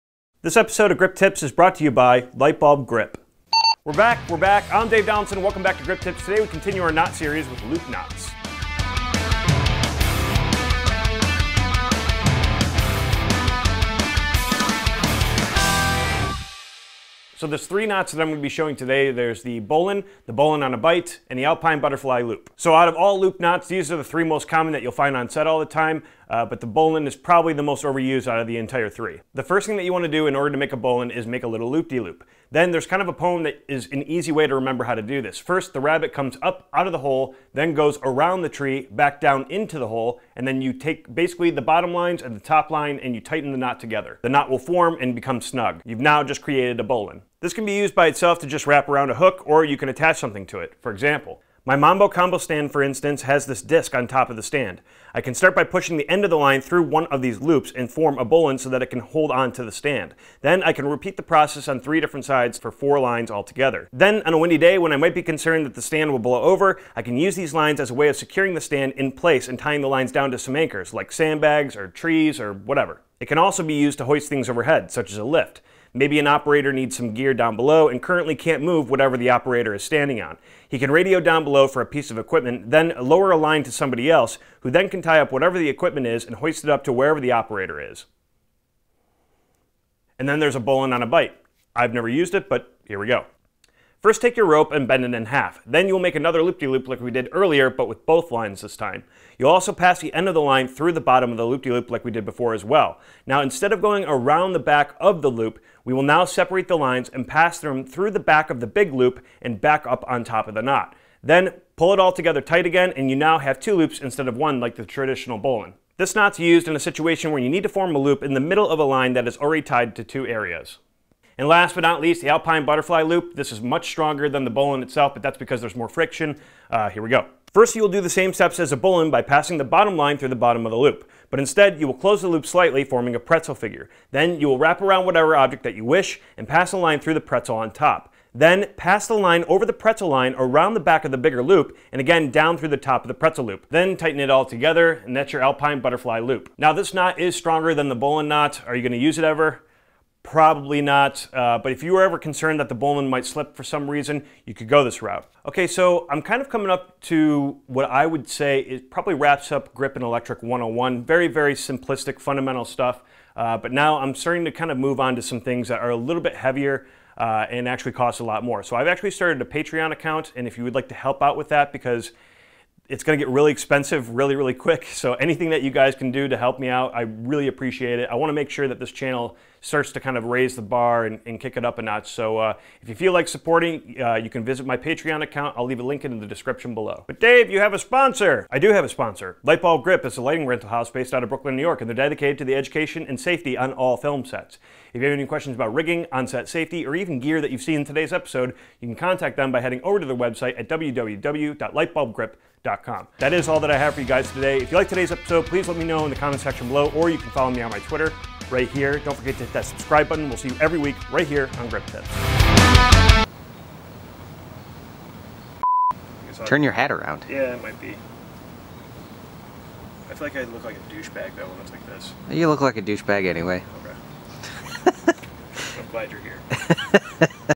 Ep 39: Loop Knots
Today we learn how to tie a Bowline, a Bowline on a Bight, and the Alpine Butterfly Loop knots

subs: 3,827